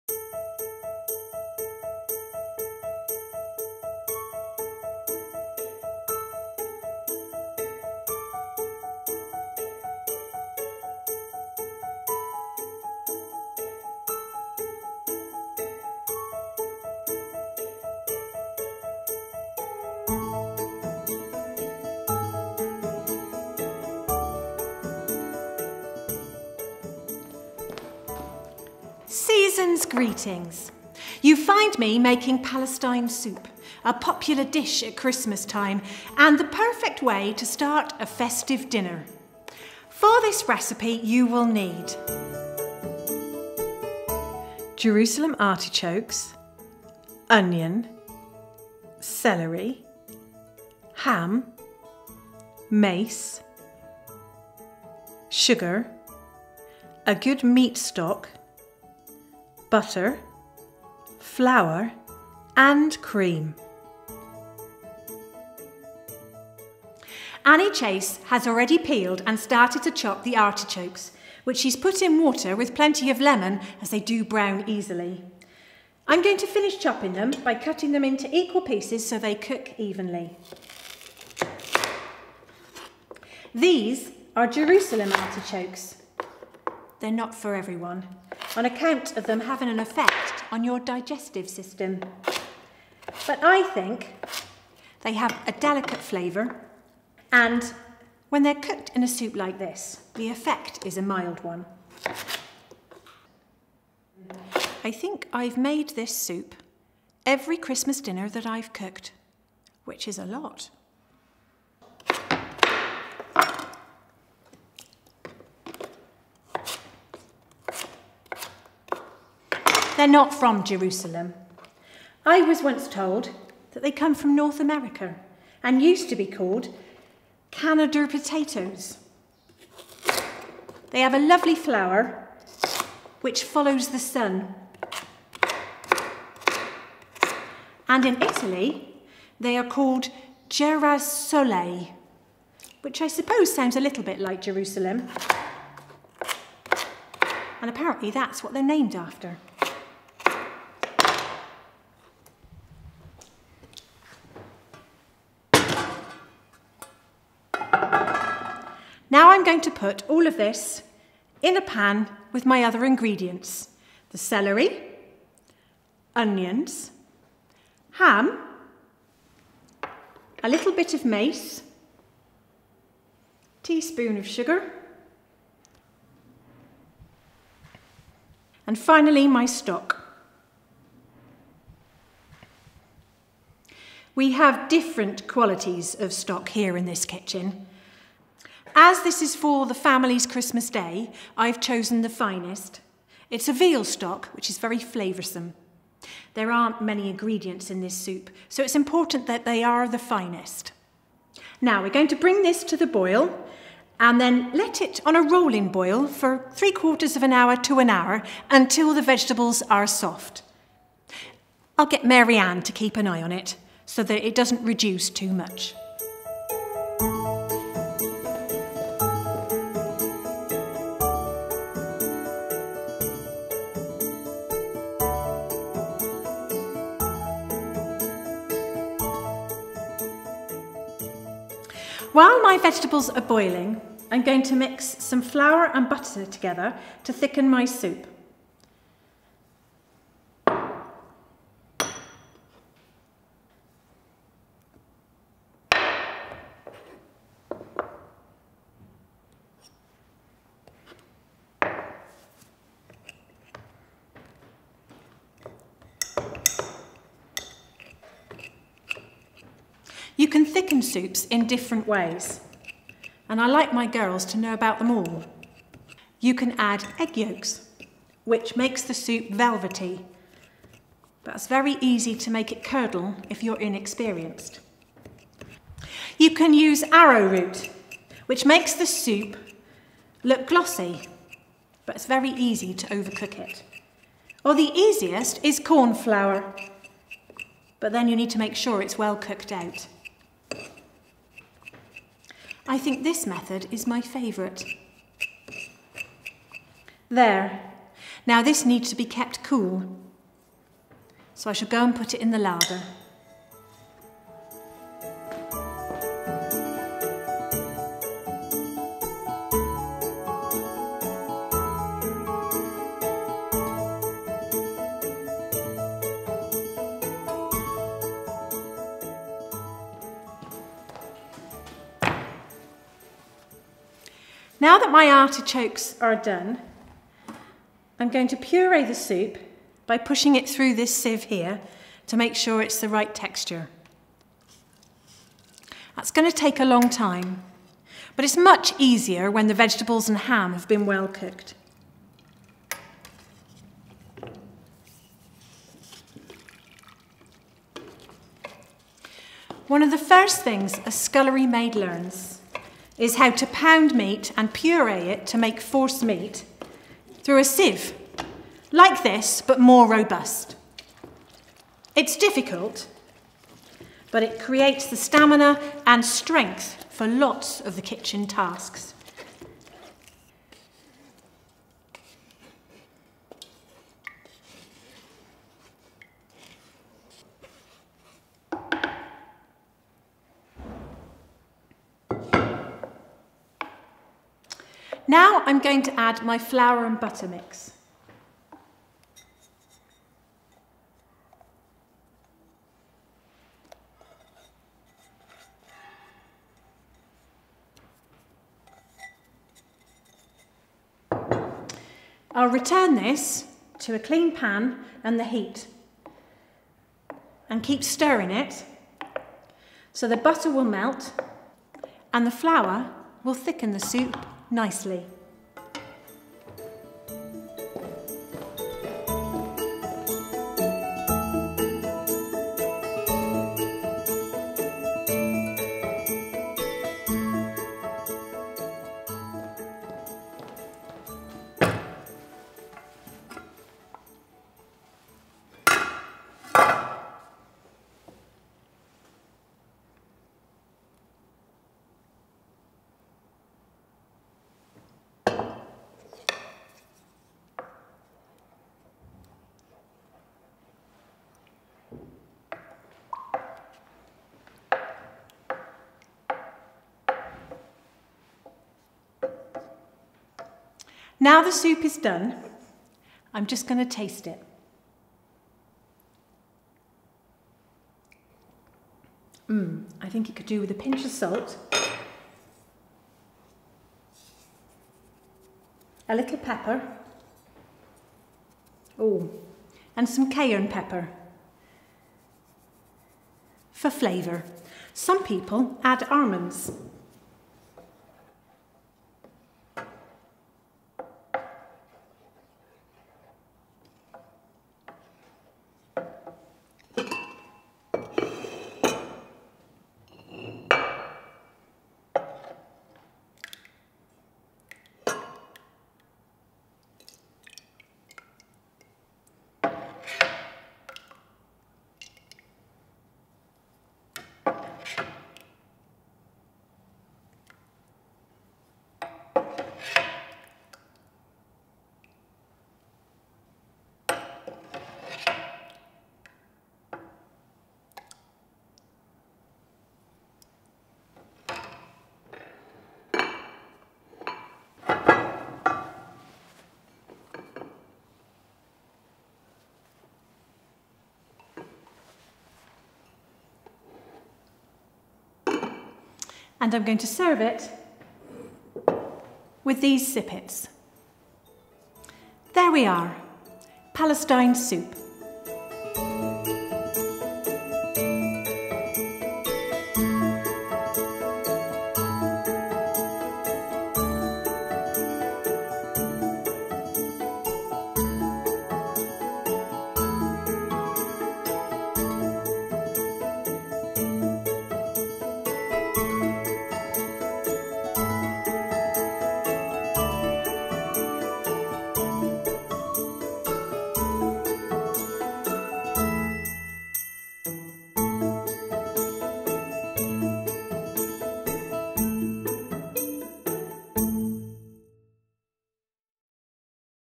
How to Make Palestine Soup — The Victorian Way
Christmastime has arrived at Audley End House, so Mrs Crocombe is preparing a traditional soup of Jerusalem artichoke, vegetables and ham — also known as 'Palestine Soup.' This light but warming winter bowl is the perfect way to start a festive dinner.

English Heritage is a charity that cares for over 400 historic buildings, monuments and sites.

INGREDIENTS
• 1.5kg / 3lb Jerusalem artichokes
• 1 onion
• 4-5 sticks celery
• 55g / 2oz ham
• 1 blade of mace (or ground mace)
• 1 heaped tsp sugar
• 1.2L / 4 UK pints chicken or veal stock
• 55g / 2oz salted butter
• 3 tbsp flour
• 300ml / 1/2 UK pint double cream
• Salt and pepper
• Cayenne pepper

METHOD
1. Cut the artichokes into small-ish equal pieces so they cook evenly.
2. Place in a pan with the diced celery, onion, ham, mace, sugar and stock.
3. Heat on a rolling boil for 45 minute to 1 hour until the vegetables are soft.
4. While the vegetables cook, mix the flour and butter in a bowl for thickening. Keep this mixture cool until ready to be used.
5. When finished boiling, puree the boiled vegetables through a sieve.
6. Add the flour and butter mix to the soup and stir thoroughly.
7. Return to the heat in a clean pan and stir until the butter and flour have mixed in fully.
8. Mix in the cream.
9. Season with more salt, pepper and cayenne to taste, then serve.

CHAPTERS
00:00 Introduction
00:39 For this recipe, you will need
01:07 Preparing the artichokes
02:38 Mixing the vegetables
03:51 Making the thickener
05:20 Sieving the mixture
06:24 Thickening the soup
07:40 Tasting and serving x